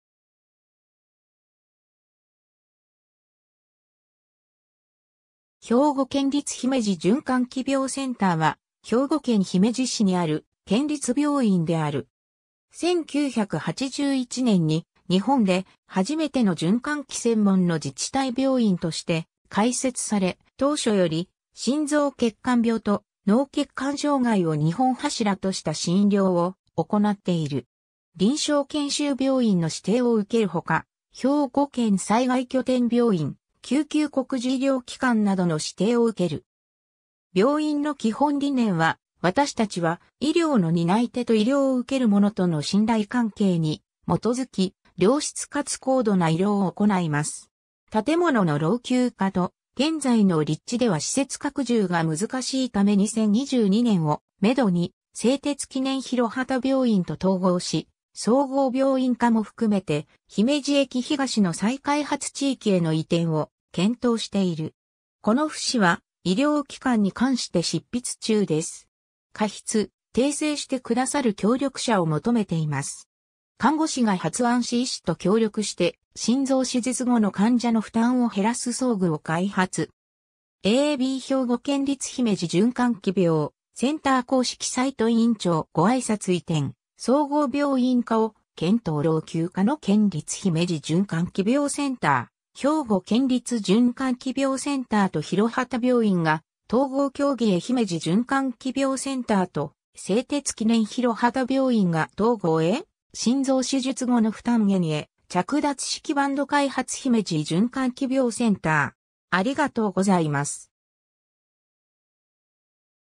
兵庫県立姫路循環器病センター
日本の医療機関_(都道府県)
兵庫県立姫路循環器病センター（ひょうごけんりつひめじじゅんかんきびょうセンター）は、兵庫県姫路市にある県立病院である。
1981年に日本で初めての循環器専門の自治体病院として開設され、当初より心臓血管病と脳血管障害を二本柱とした診療を行っている。
臨床研修病院の指定を受けるほか、兵庫県災害拠点病院、救急告示医療機関などの指定を受ける。
病院の基本理念は、「わたしたちは、医療の担い手と医療を受けるものとの信頼関係に基づき、良質かつ高度な医療を行います。」 建物の老朽化と、現在の立地では施設拡充が難しいため2022年をめどに製鉄記念広畑病院と統合し総合病院化も含めて姫路駅東の再開発地域への移転を検討している。
 この節は医療機関に関して執筆中です。
加筆、訂正して下さる協力者を求めています（PJ医療機関）。
 看護師が発案し医師と協力して心臓手術後の患者の負担を減らす装具を開発。
 ^ a b 兵庫県立姫路循環器病センター公式サイト 院長ご挨拶 ^ 移転、総合病院化を検討　老朽化の県立姫路循環器病センター ^ 兵庫県立循環器病センターと広畑病院が統合協議へ ^ 姫路循環器病センターと製鉄記念広畑病院が統合へ ^ 心臓手術後の負担減へ着脱式バンド開発　姫路循環器病センター